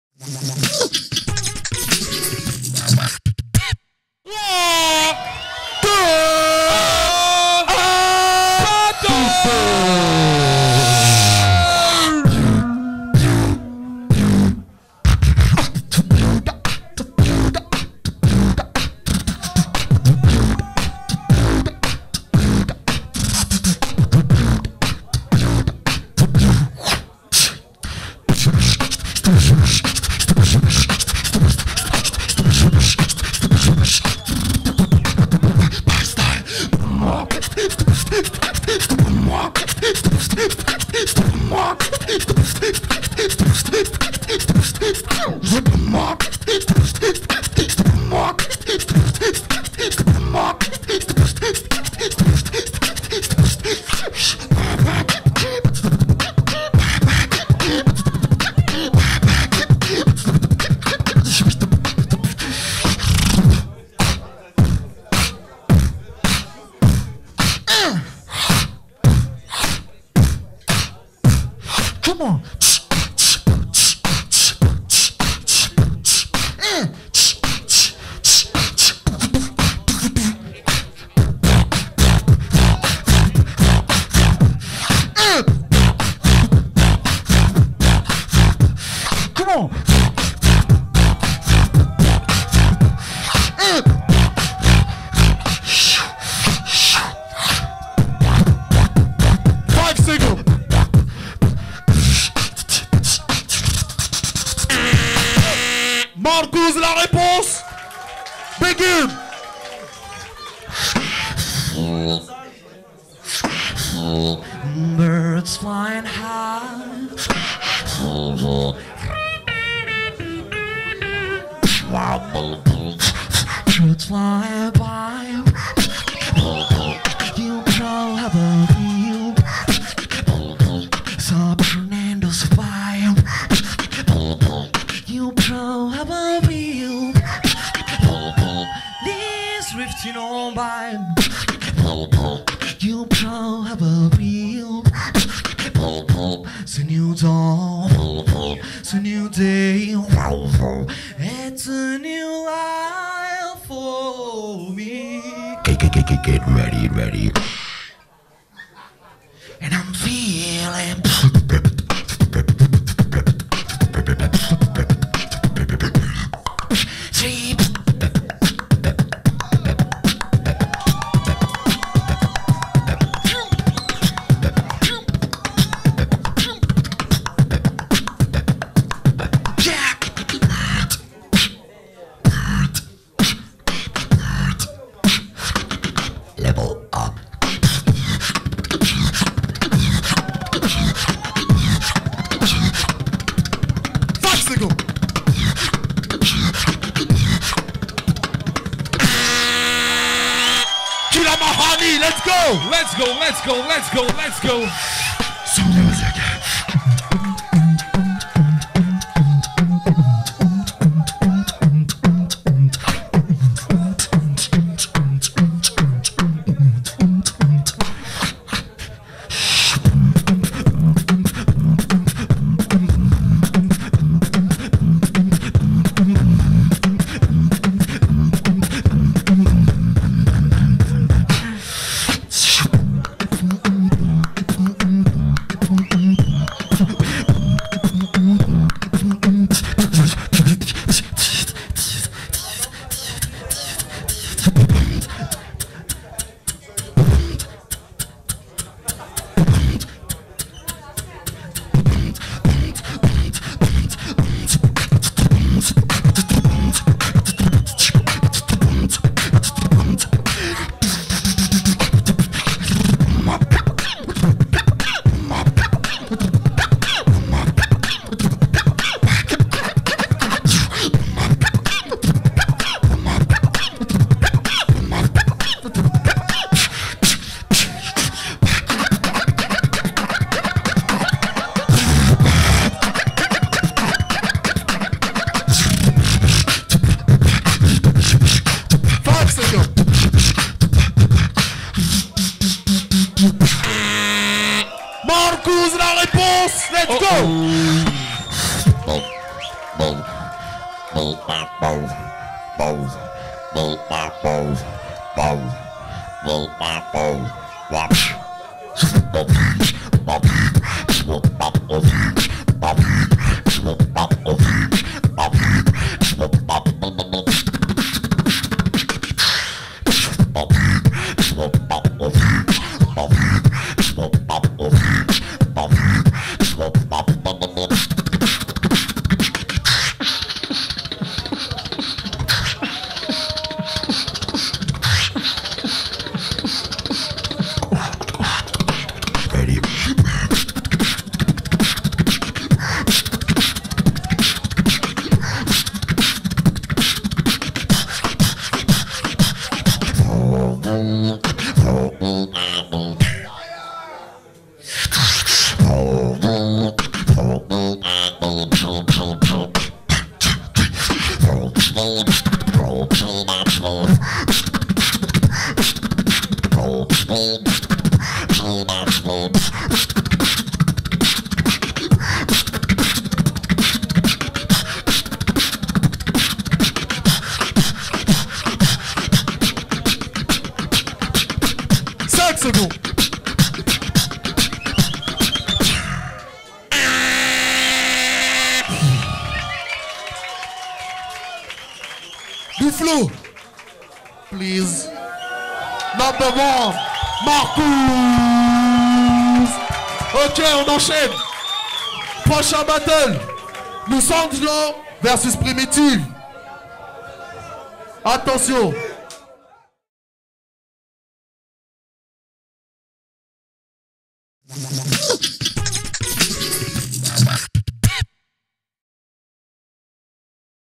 LA CUP | Markooz (ESP) VS Killa Mahanie (BEL) | Quarter Final
LA CUP 2013: Spain vs Belgium - Markooz vs Killa Mahanie
Spain: 15 Points - Belgium: 0 Points

how does the point system work:

0 points for a defeat
1 point for a draw
3 points for a win in solo
5 points for a win in team

each judge is able to give away full points to a team member.

Judges were Felix Zenger (Fin), Pepouni (CH), Skiller (Bul), Slizzer (Lux) & ZeDe (CH)

Swissbeatbox.com